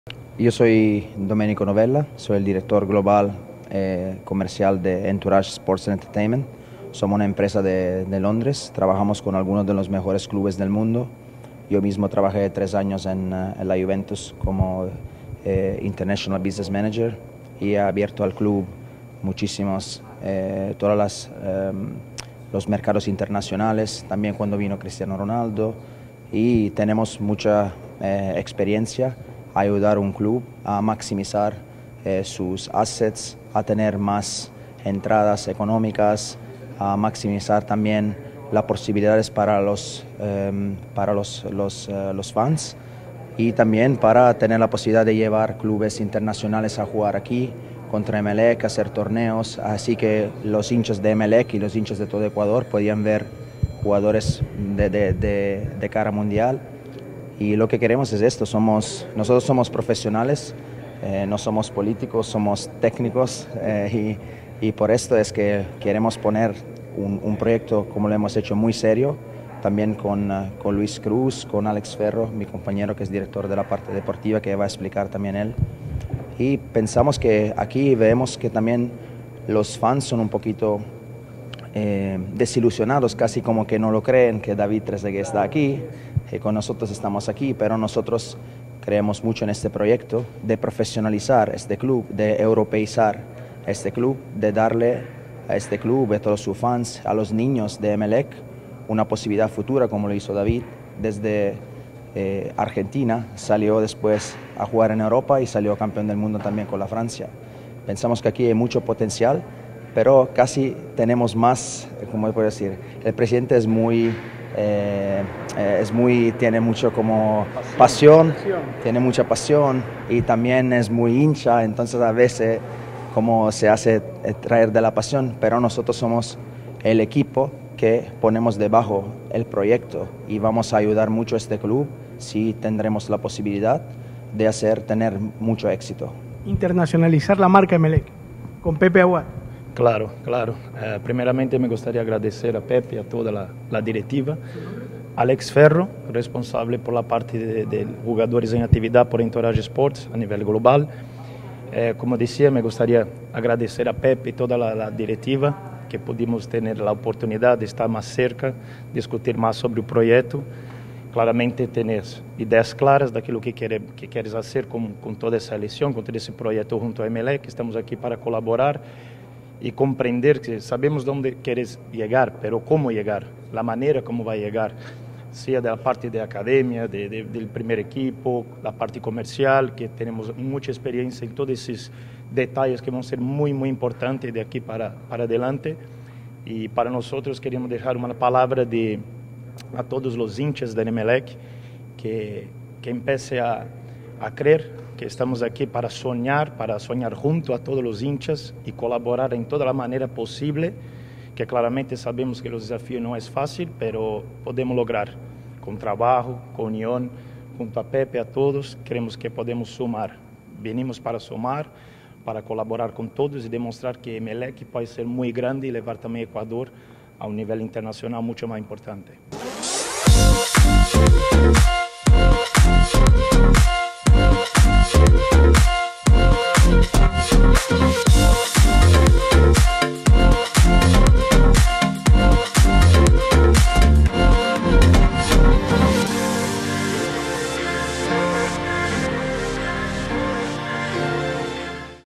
QUIÉN ES DOMENICO NOVELLA - EMPRESARIO EUROPEO CON PROYECTO PARA EMELEC 2022
El empresario italiano, Doménico Novella, conforma parte del equipo de David Trezeguet y plantean proyecto para hacer gira de partidos internacionales con Emelec posicionando la marca en el mercado mundial.